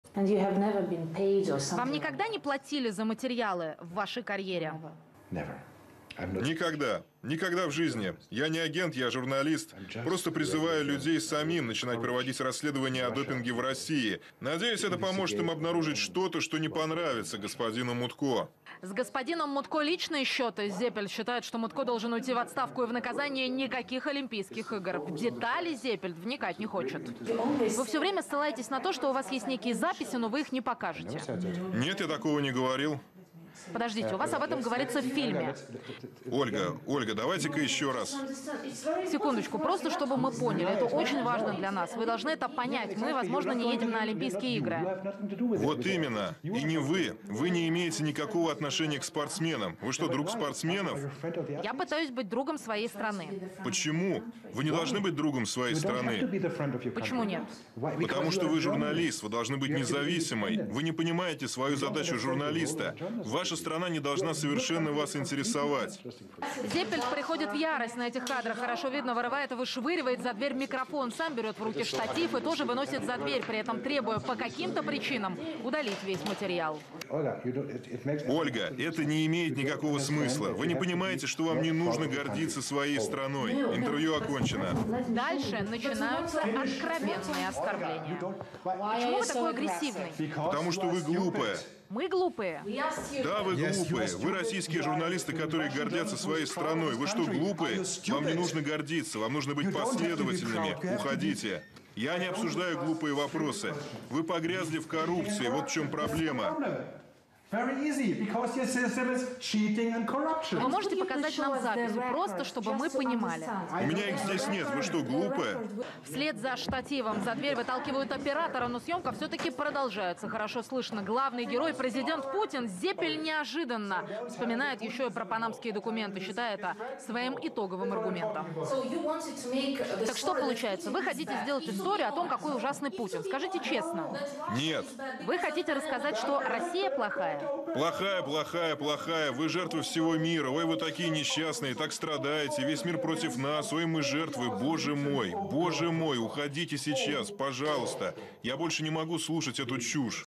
Německý novinář Hajo Seppelt ukazuje podstatu RU žurnalistiky (2016) - CZ titulky
Překlad: "A nikdy jinak!"